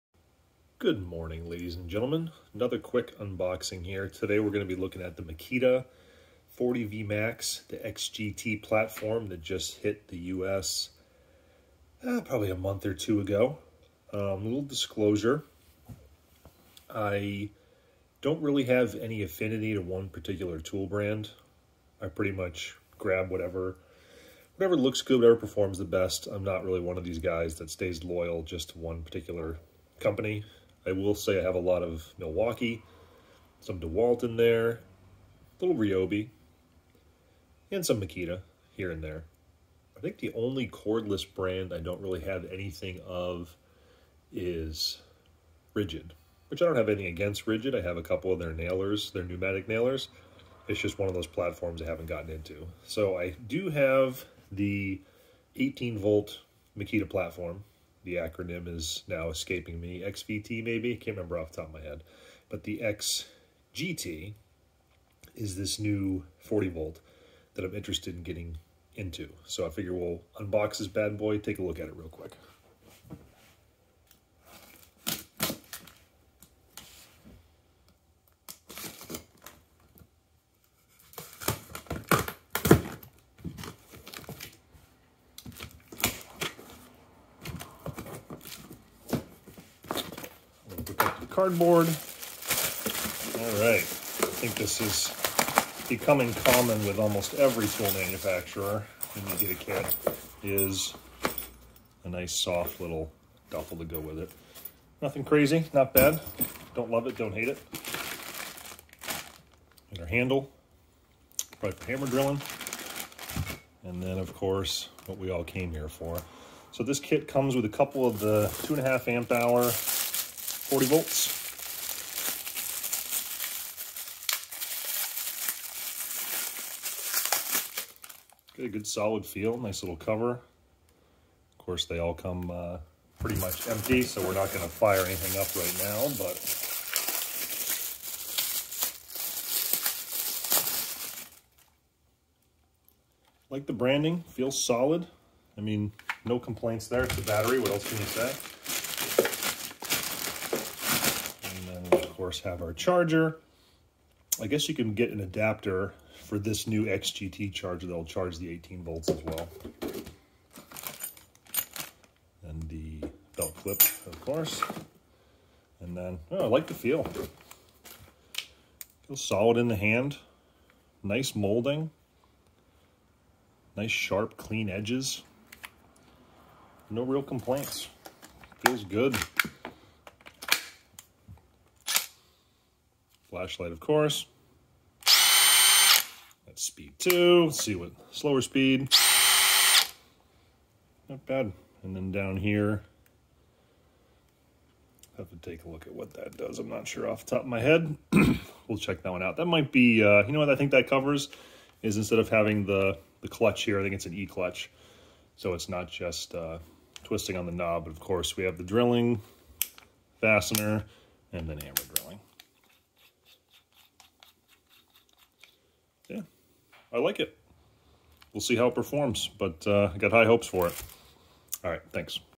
Makita XGT 40v Cordless Hammer Drill / Driver (GPH01): Unboxing and Initial Impressions
A real quick and dirty unboxing of Makita's new 40v XGT cordless hammer drill / driver (GPH01). The XGT line recently launched in North America and I was in need of a new drill/driver. I generally don't stick with one tool brand or fanboy for a single company. I don't mind swapping between manufacturers. I will say that I predominantly own Milwaukee tools and DeWalt in a close second, but I also have Makita, Ryobi, and even some Metabo sprinkled in there.